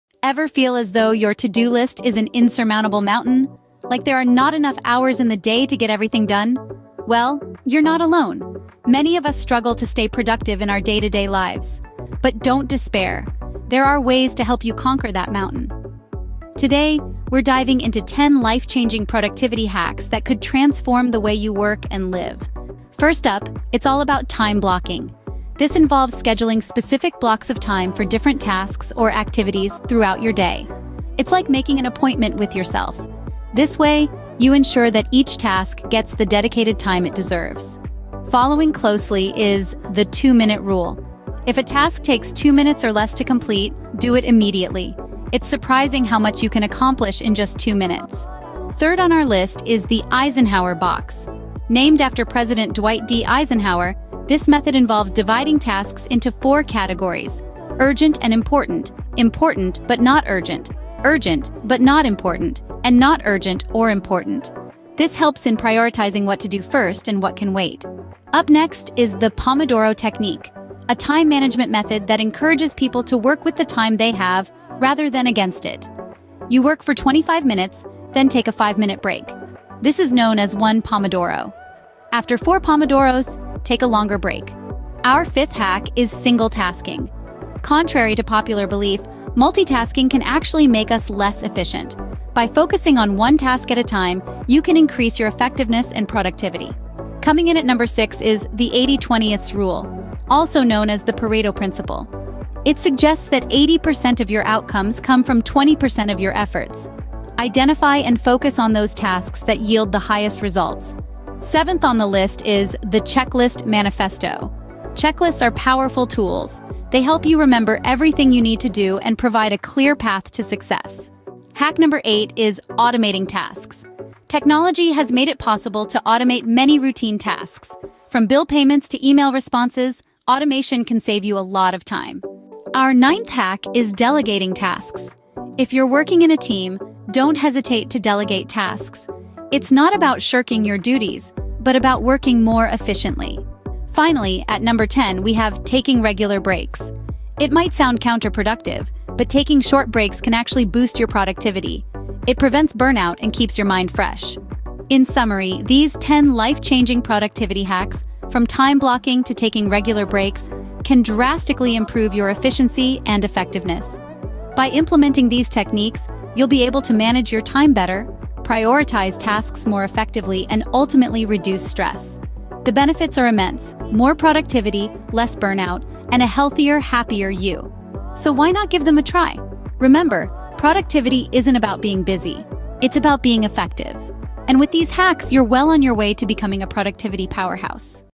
🚀 10 Game-Changing Productivity Hacks to Transform Your Life! 🌟 Boost Efficiency and Crush Goals! 💪
00:00:00 Introduction
00:00:22 Time Blocking
00:00:37 The Two Minute Rule
00:00:48 The Eisenhower Box
00:01:08 The Pomodoro Technique
00:01:24 Single-Tasking
00:01:36 The 80/20 Rule
00:01:51 The Checklist Manifesto
00:02:01 Automating Tasks
00:02:12 Delegating Tasks